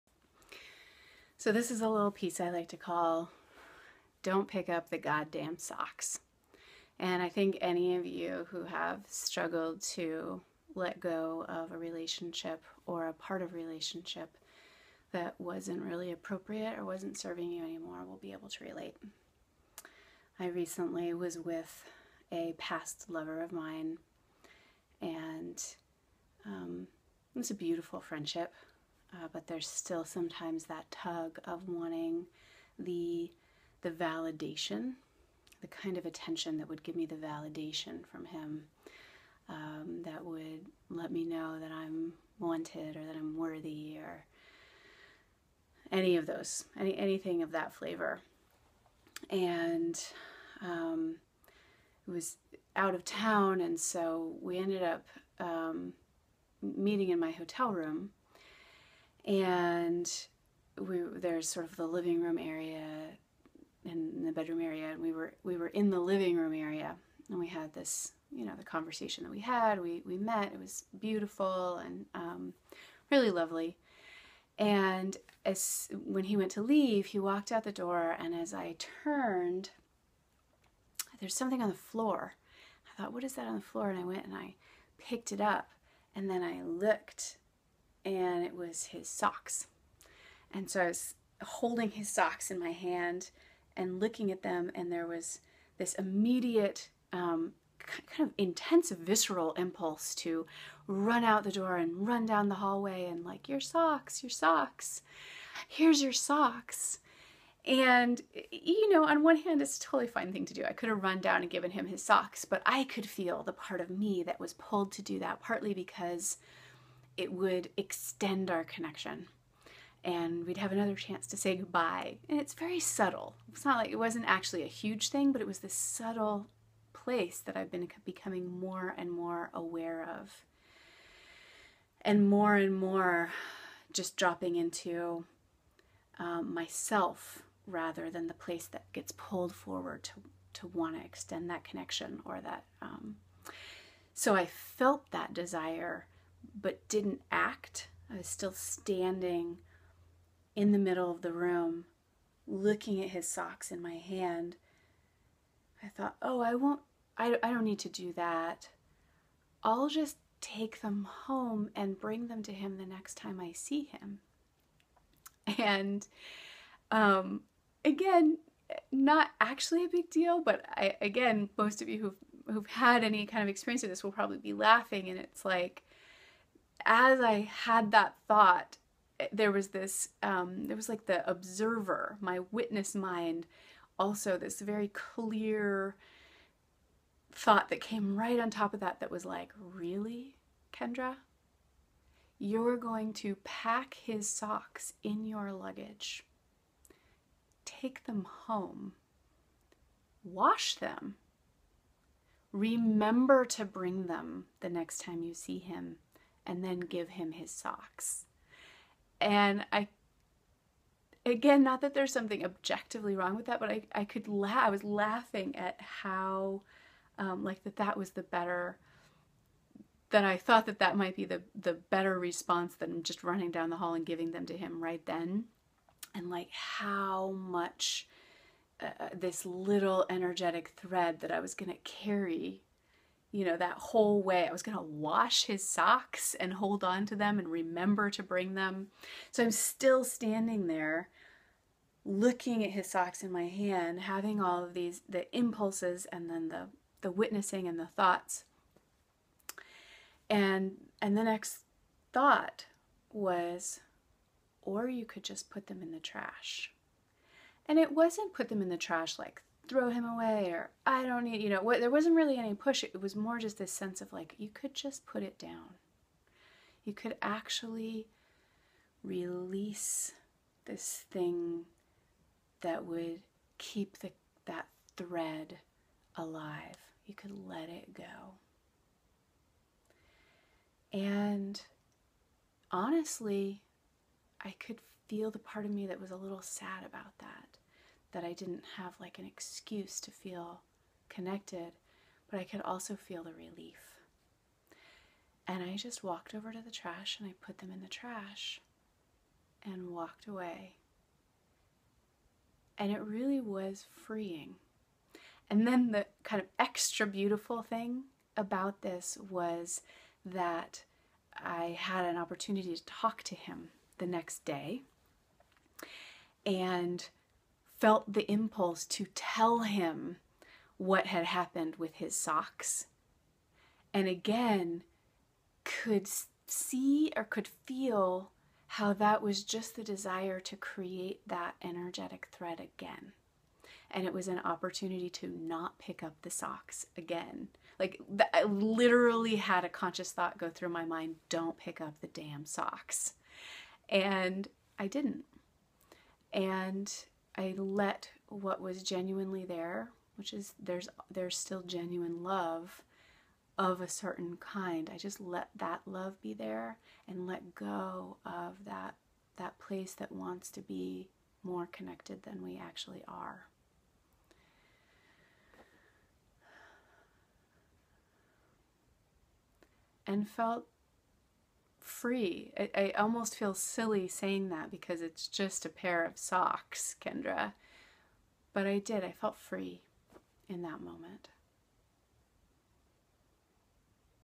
Don't Pick Up the {Damn} Socks!!
There's a lot of talk about 'letting go' in relationships, but not so much about what that actually means, or HOW to do it.
In this video, I share a personal story of the moment-to-moment practice of letting go & how free I felt on the other side.